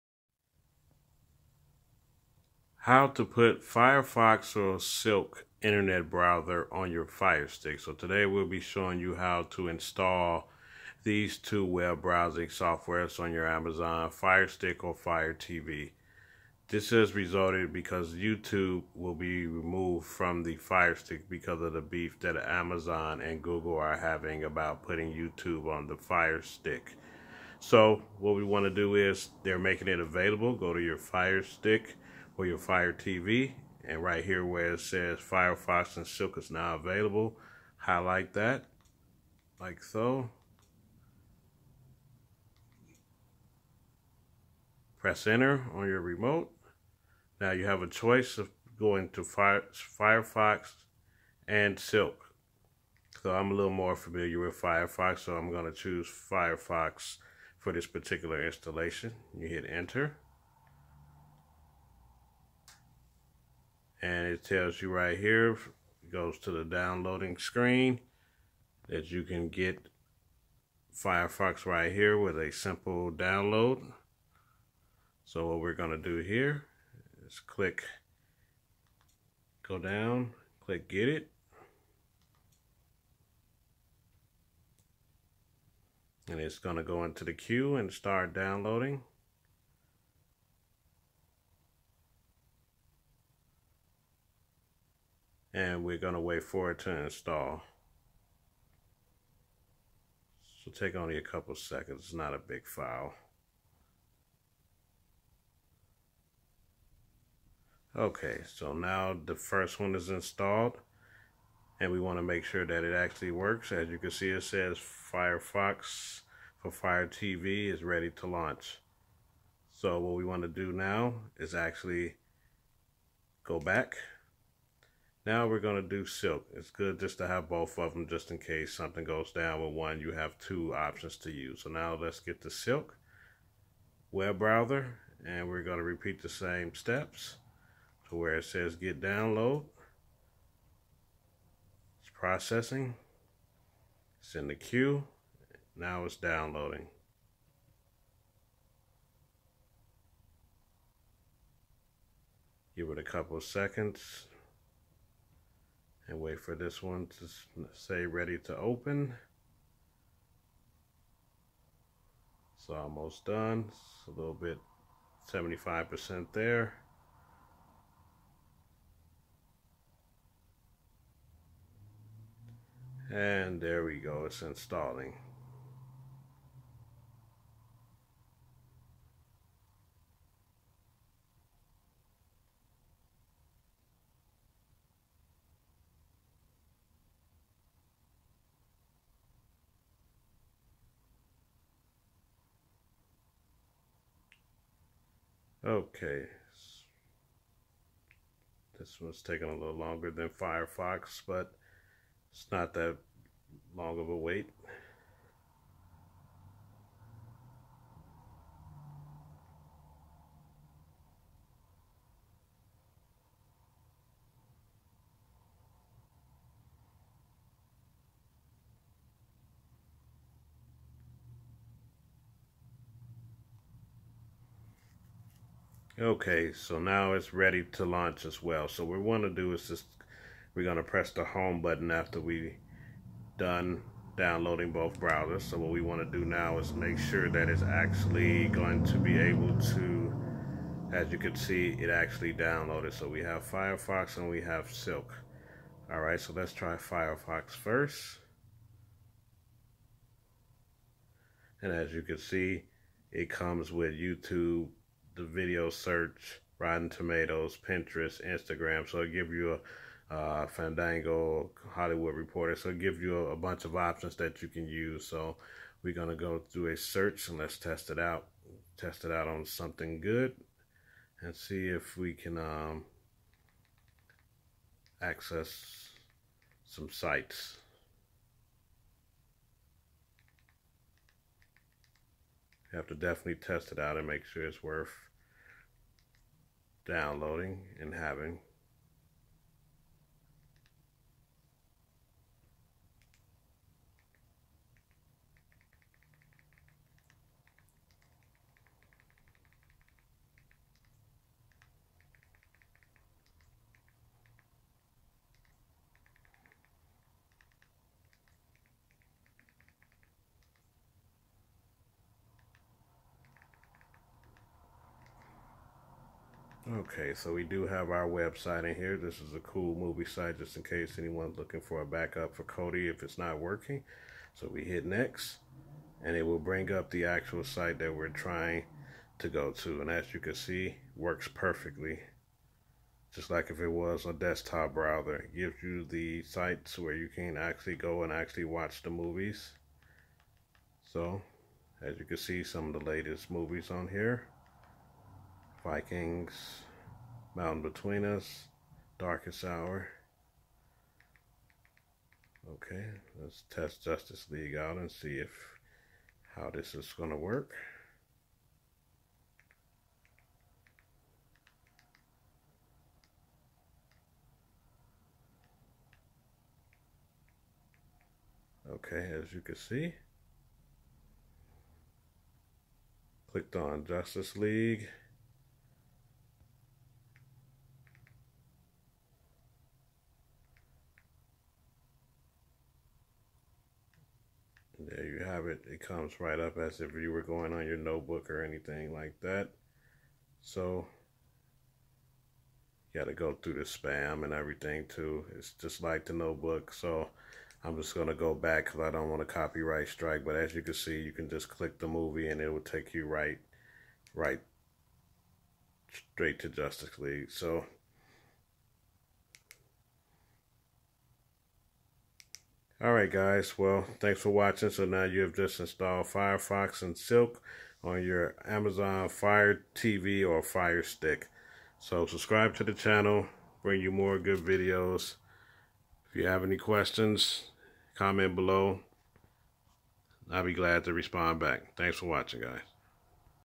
How to install Fire Fox and Silk Web Browser on your Fire TV Stick
This is a tutorial to show you how to install Firefox Web browser and silk Web browser on to your fire TV or fire TV stick. You too will soon be removed from Amazon fire stick because of the ongoing few between Google And Amazon.